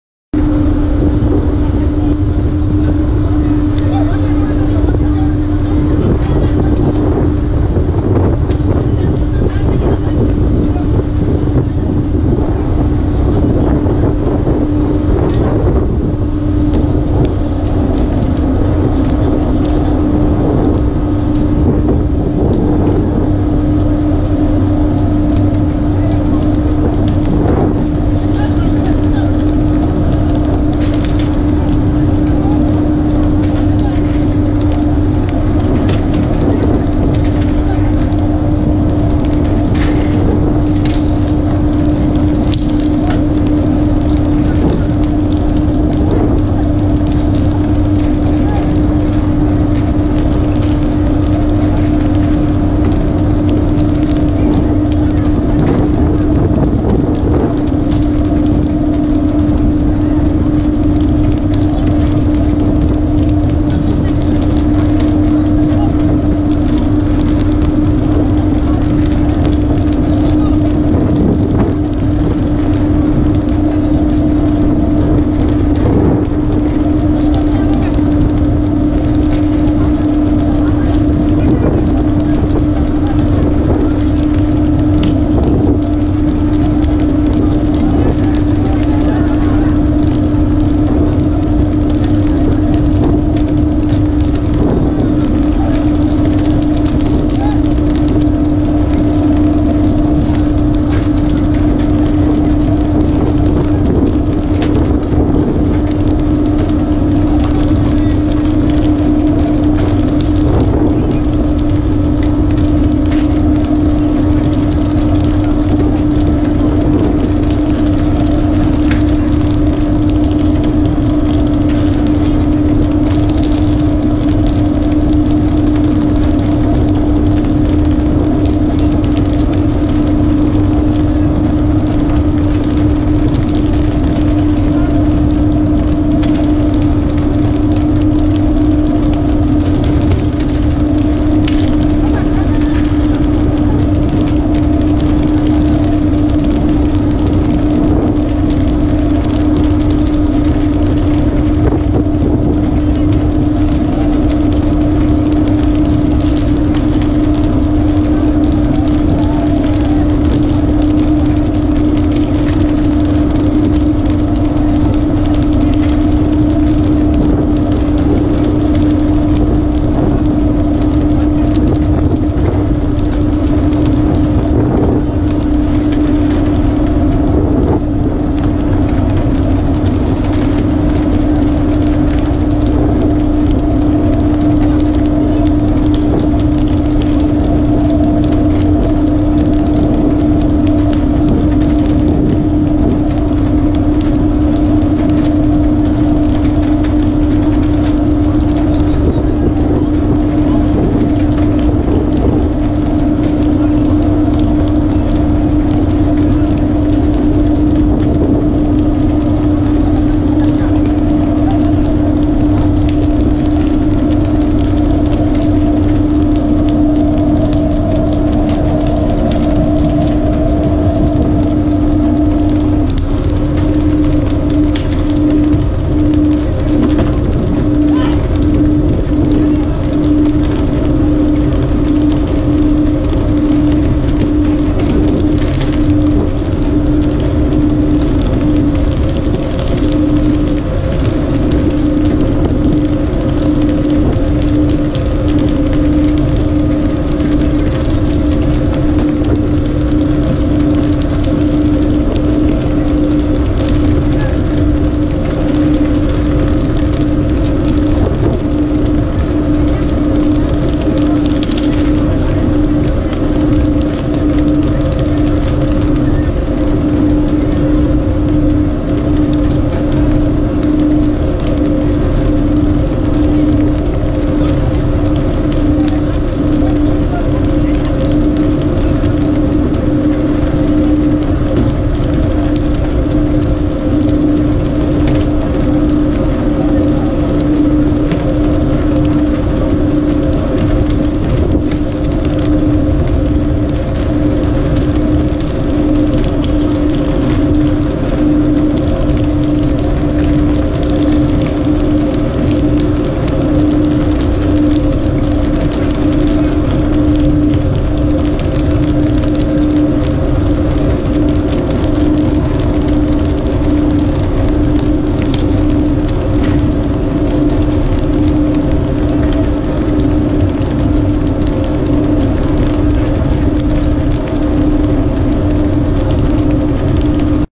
Maldives_fishing_a_special_moment_🥰🥰🥰 (240p)
Fishing scene in the sea country Maldives